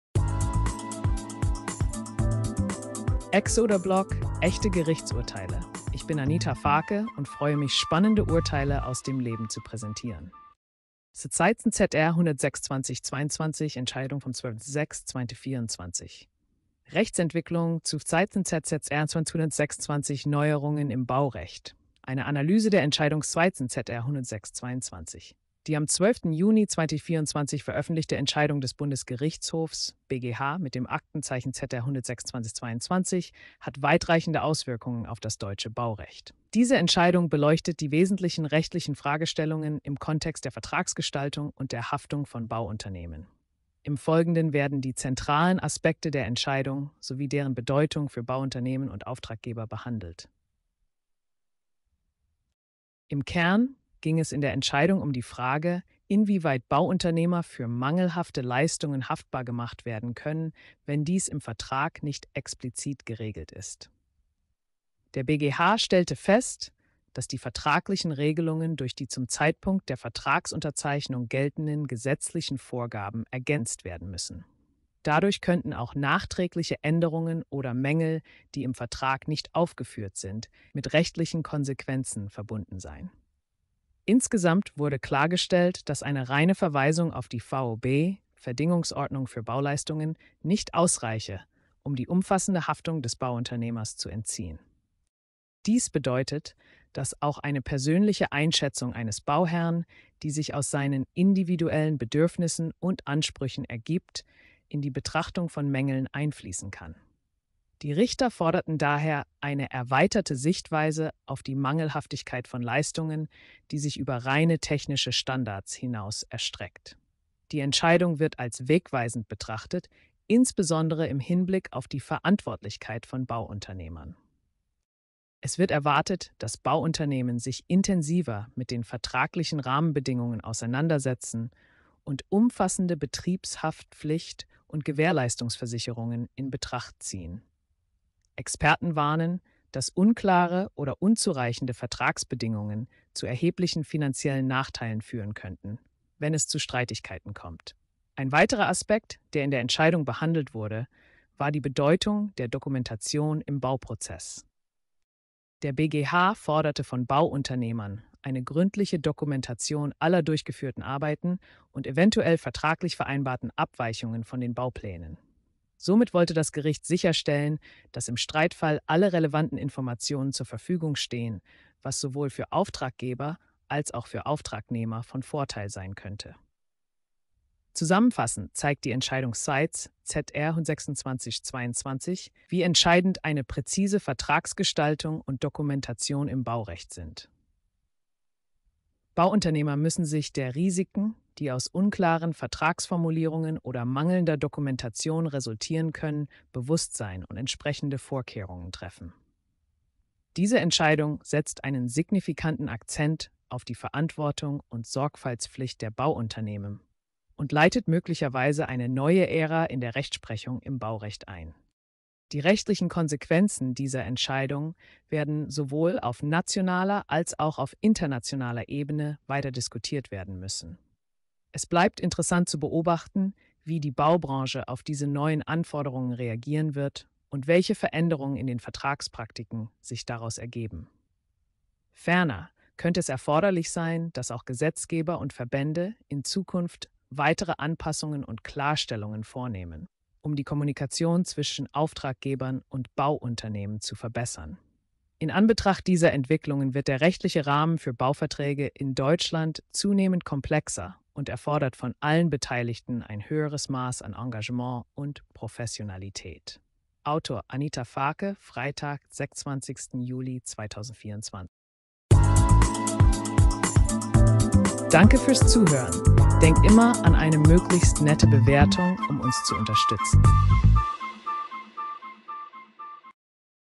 VII ZR 126/22, Entscheidung vom 12.06.2024 Neuerungen im Baurecht (Podcast)
Die Entscheidung VII ZR 126/22 des Bundesgerichtshofs, veroeffentlicht am 12. Juni 2024, hat erhebliche Auswirkungen auf das deutsche Baurecht. Sie thematisiert die Haftung von Bauunternehmen fuer mangelhafte Leistungen, auch wenn diese nicht im Vertrag aufgefuehrt sind. Der BGH stellte fest, dass vertragliche Regelungen durch geltende gesetzliche Vorgaben ergaenzt werden muessen, was die Verantwortung der Bauunternehmer erhoeht. Zudem wird die Definition von Maengeln erweitert, indem subjektive Erwartungen der Bauherren beruecksichtigt werden. Die Entscheidung unterstreicht die Wichtigkeit praeziser Vertragsgestaltung und umfassender Dokumentation im Bauprozess, um rechtlichen Konflikten vorzubeugen und die Risiken fuer alle Beteiligten zu minimieren. AI-generated content